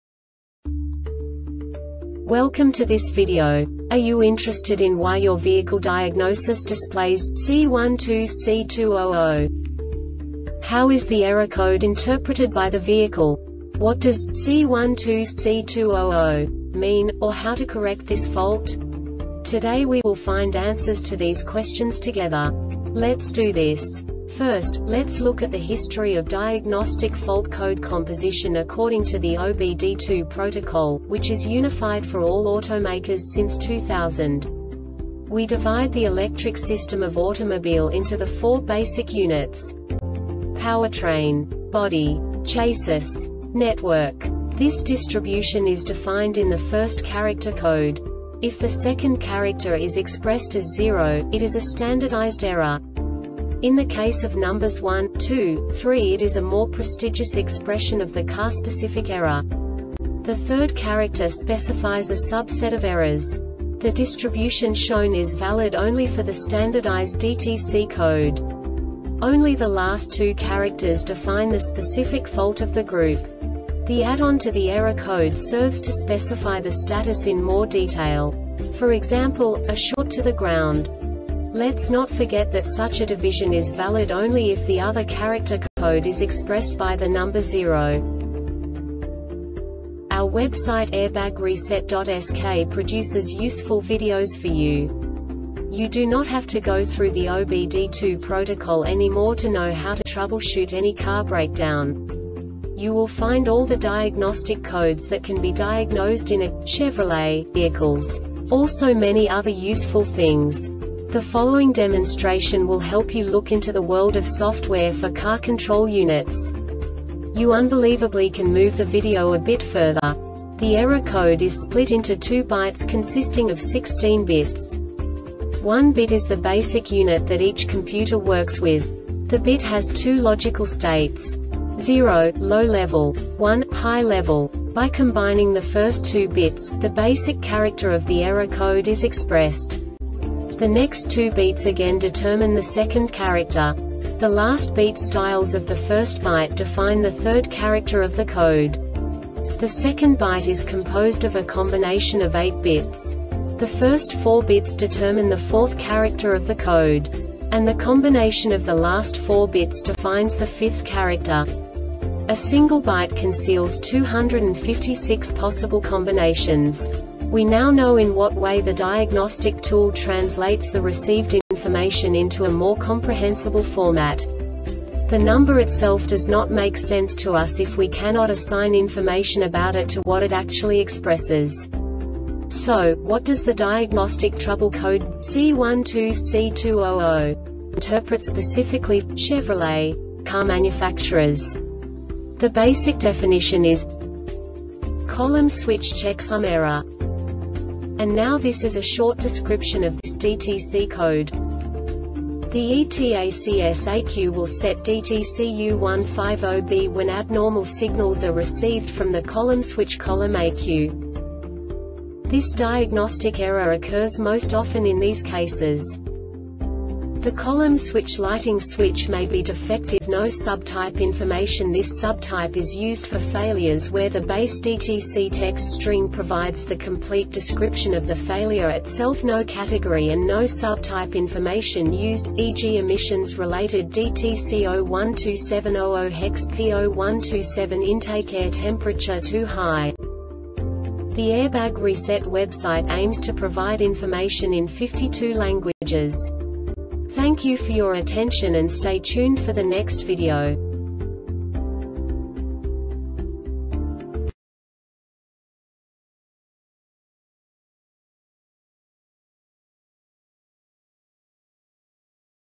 DTC Chevrolet C12C2-00 Short Explanation
Contents:
0:21 Basic DTC analysis according to OBD2 protocol standard.
1:48 Insight into programming
2:58 A diagnostically understandable description of the Diagnostic Trouble Code
3:05 Basic error definition

Make:
Chevrolet
Code:
C12C2 00
Definition:
Column Switch (checksum error)
Description:
The ETACS-ECU will set DTC U150B when abnormal signals are received from the column switch (column-ECU).
Cause:
 The column switch (lighting switch) may be defective.